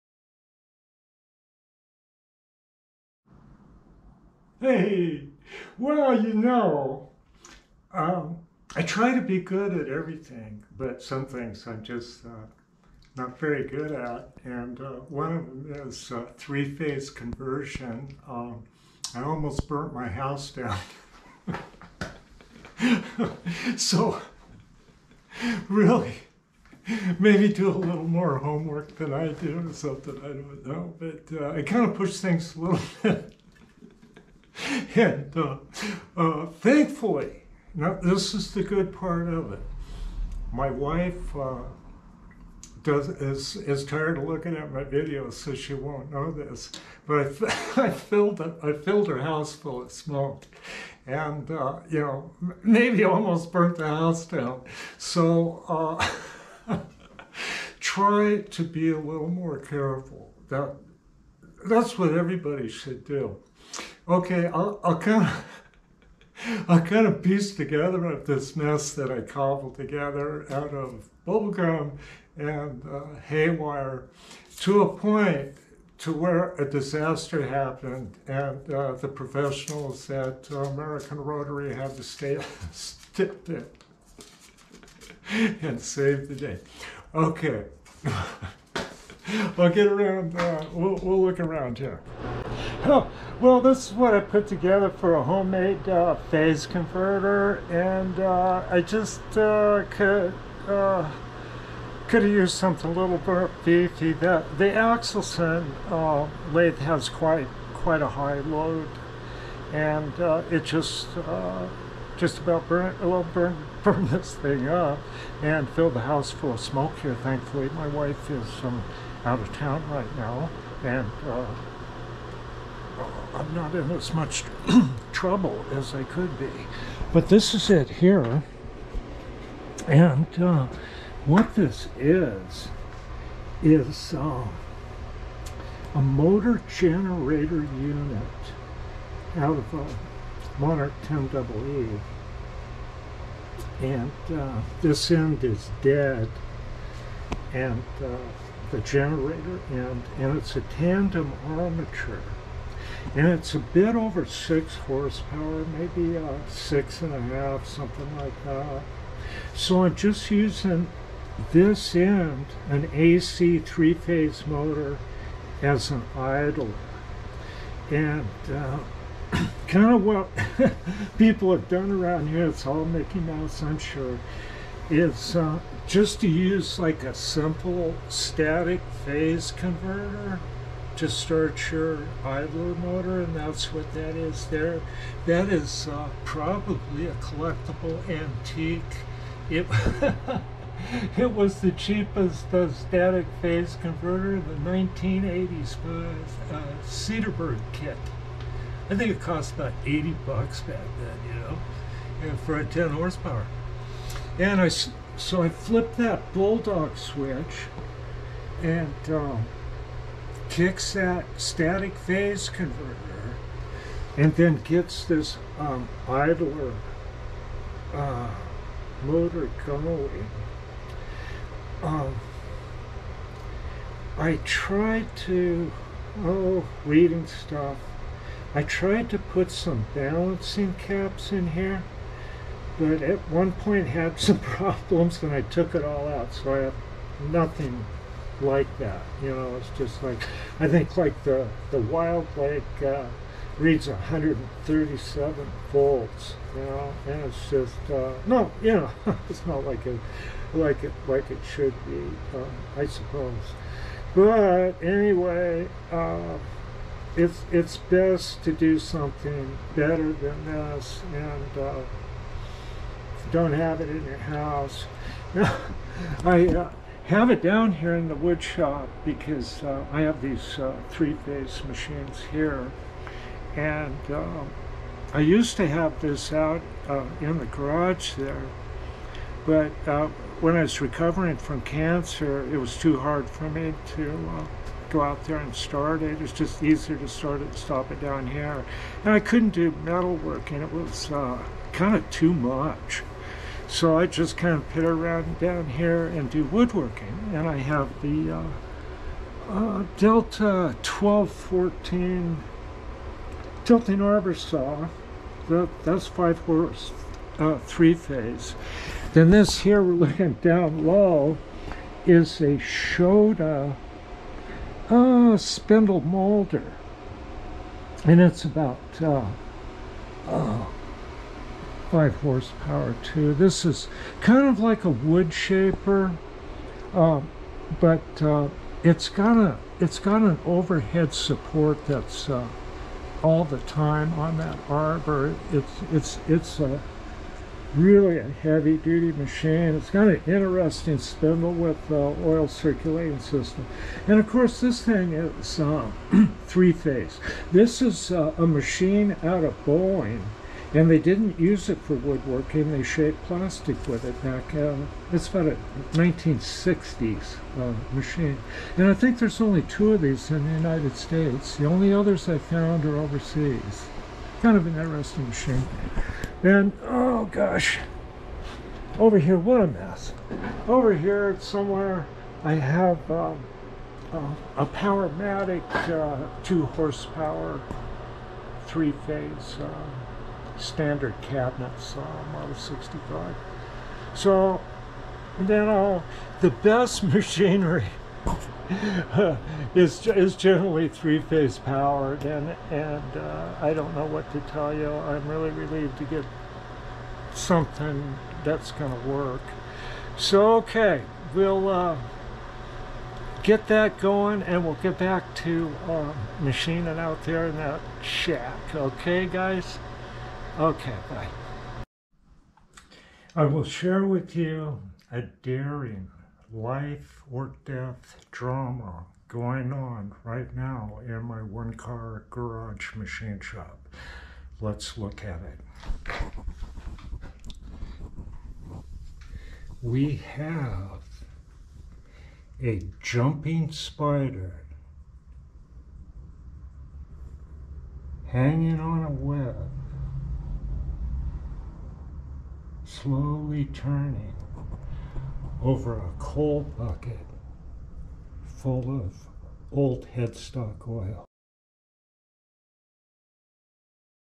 Bubble Gum + Hay Wire Phase Conversion 101
Behold Me the worst phase converter hack on YouTube! But it is alright my wife is out of town! American Phase Converter is going to make it possible to continue making these unusual machine shop videos. Better news for Charlie the little white dog, he is at the Merry Widow Mine in Montana and doing good.
The technical facts of what happened
The Axelson Lathe overloaded my own Gomer Pyle designed system of ancient junk, I do know better.
The center contact in the also ancient Axelson motor starter had iffy contact anyway caused one of the mains wires to overheat, and being over a part of the switch it shorted tripping my house breaker.
I reset the house breaker that is just outside, went back in the shop to try to start the lathe.
Not exactly sure here but, the phase converter did not start and sat there and single phased or buzzed not turning until it burnt up windings while I was dinking around in the shack, I saw smoke coming from the basement in the house!
 Lesson learned, I feel like an idiot!